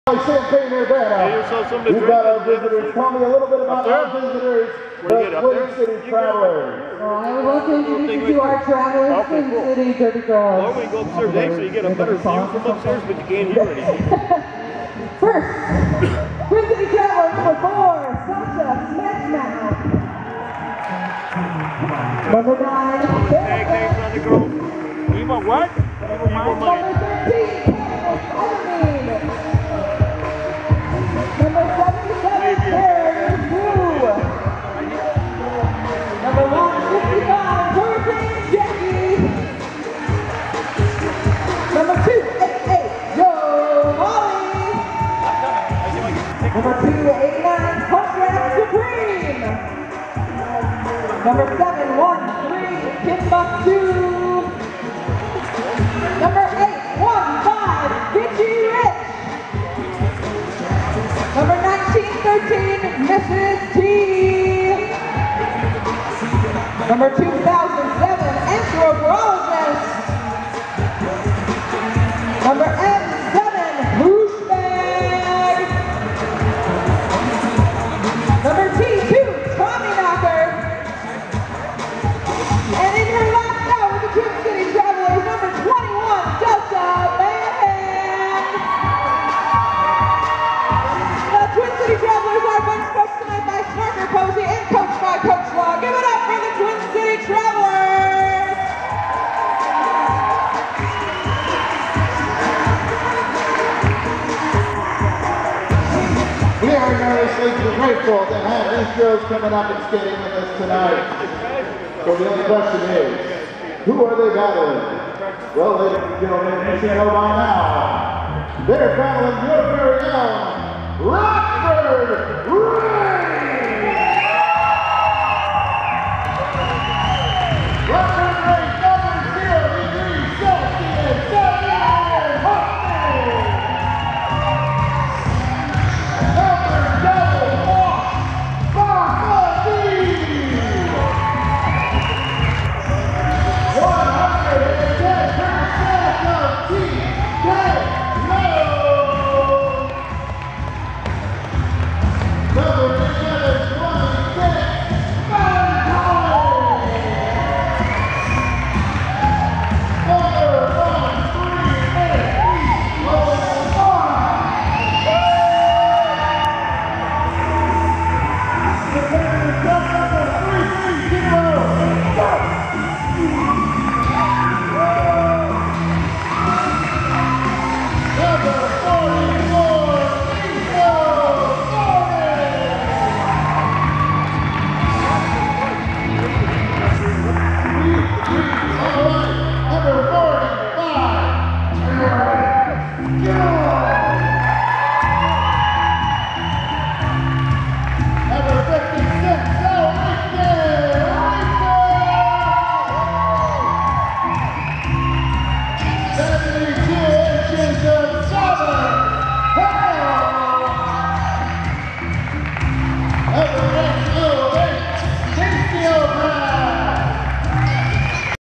Roller Derby Night: Rockford Rage Vs Twin City Derby Girls
The team introductions